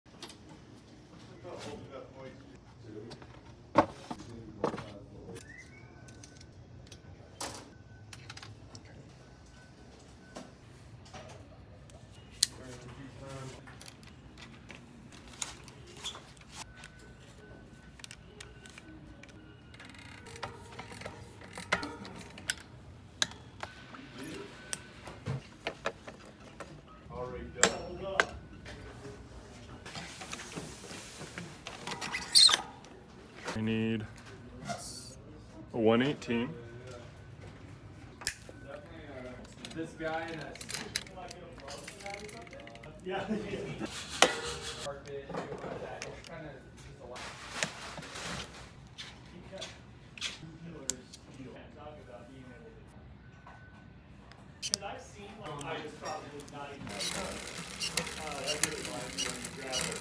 SRAM Transmission Installation
We heard you can stand on the derailleur, but why would you want to? 🤷 Our favorite feature of the new SRAM Transmission is how quickly it goes from in a box to on a bike! Don’t believe us, just watch. 👀